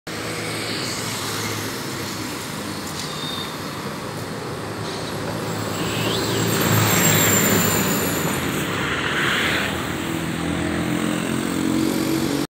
📸rodoviária bento são Tomé na empresa Viamão de transporte coletivo Ltda n_°8312 no vídeo feito poa
📸@guicapilheira53 📸rodoviária bento são Tomé na empresa Viamão de transporte coletivo Ltda n_°8312 no vídeo feito em Porto Alegre Rio Grande do Sul Av Bento Gonçalves paternon
observação:🤙 SE GOSTARAM DEIXA TAMBÉM LÁ -LOS COMENTÁRIOS OK É NÓS TMO JUNTO SEMPRE MOVIMENTO DO TRANSPORTE COLETIVO METROPOLITANO RS